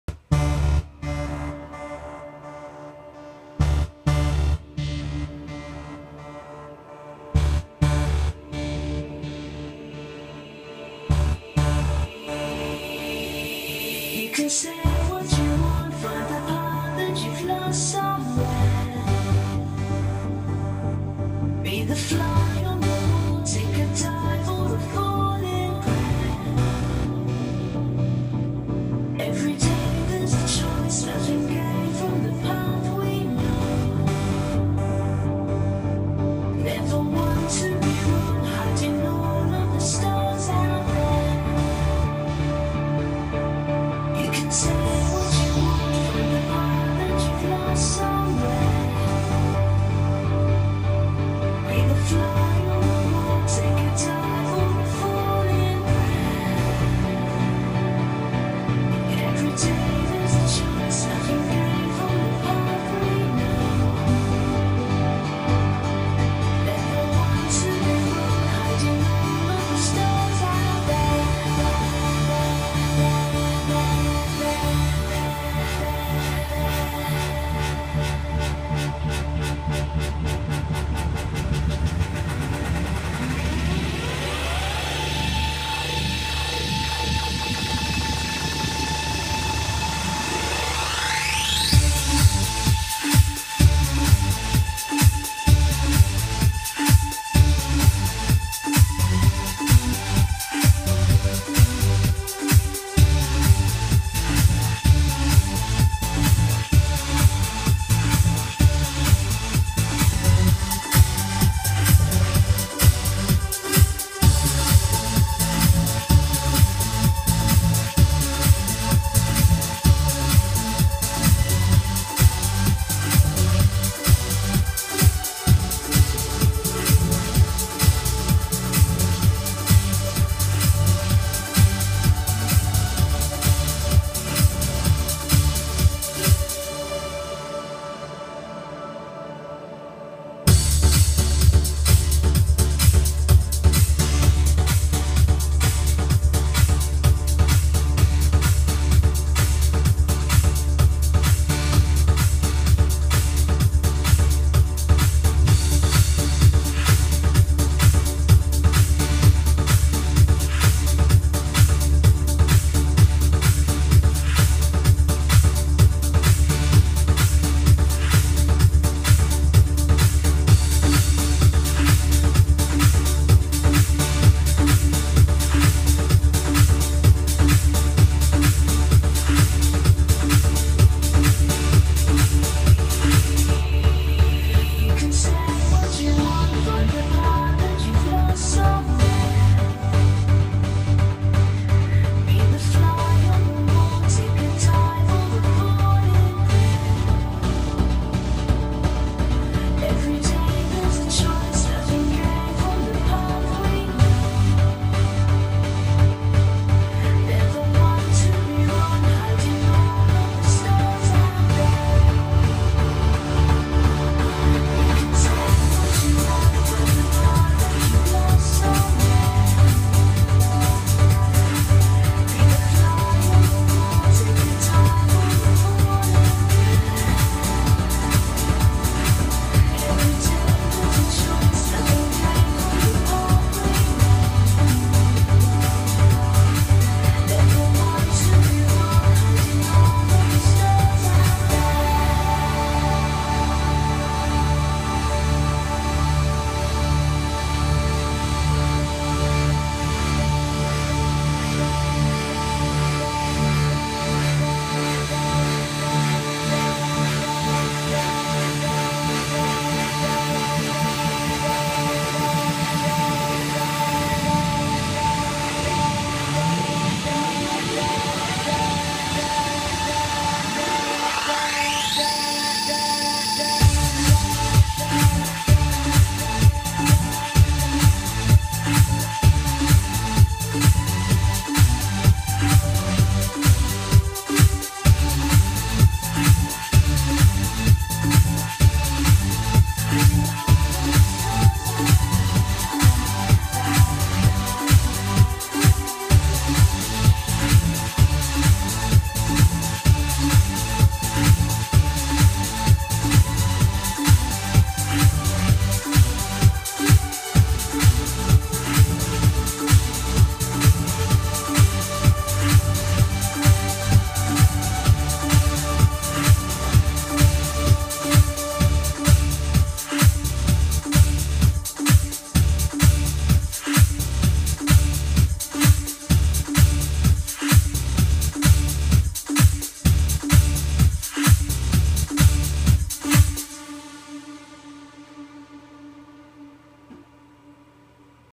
Hiding all the stars
Scenes and animations for the show are provided by Pangolin Quick Show 'Cloud' users. Most scenes have been altered, re aligned, or changed for multiple projectors. There are (2) Tarm Two projectors used for this show.